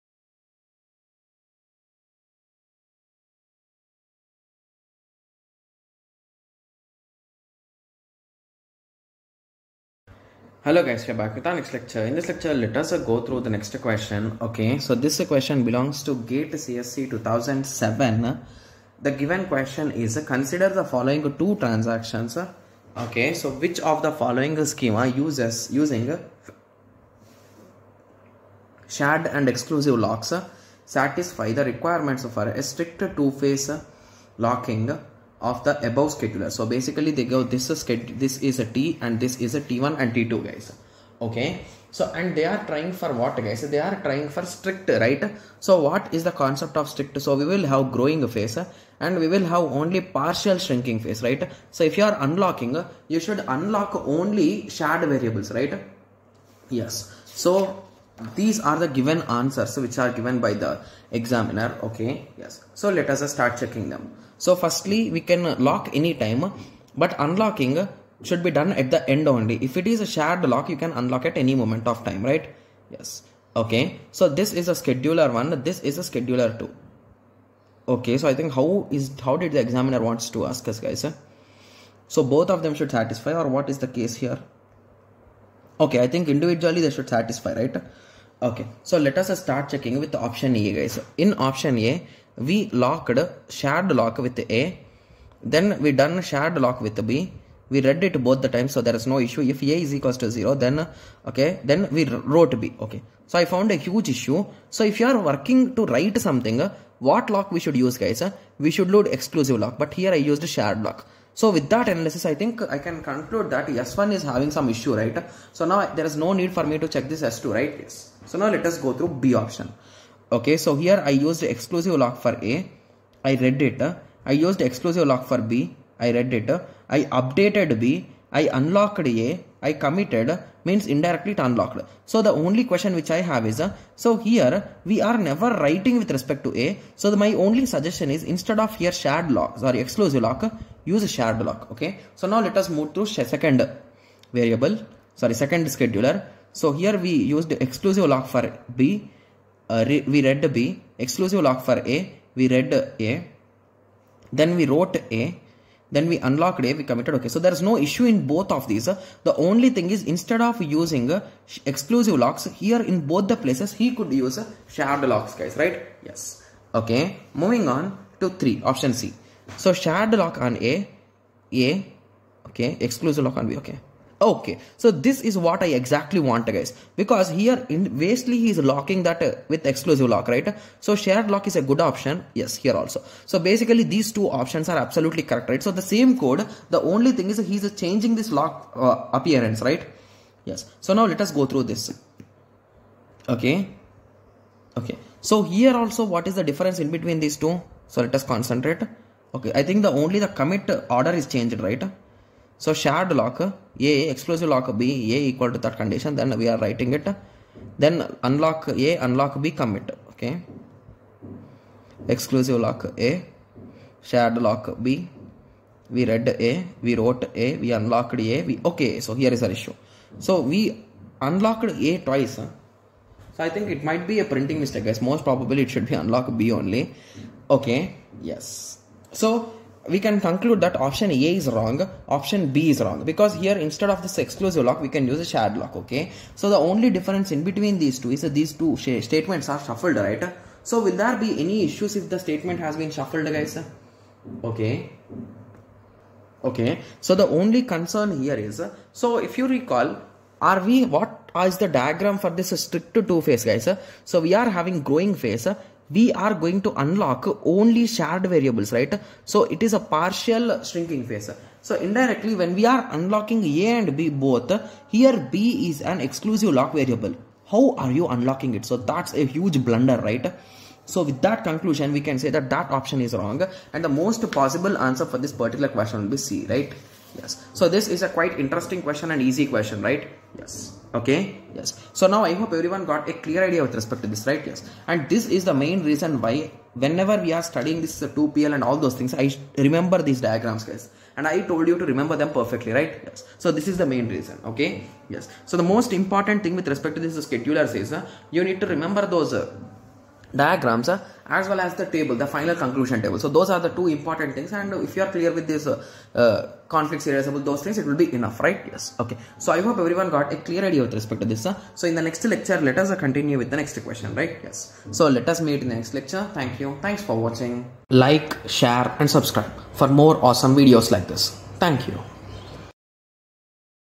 GATE CSE 2007
Consider the following two transactions : T1 and T2.Which of the following schemes, using shared and exclusive locks, satisfy the requirements for strict two phase locking for the above transactions?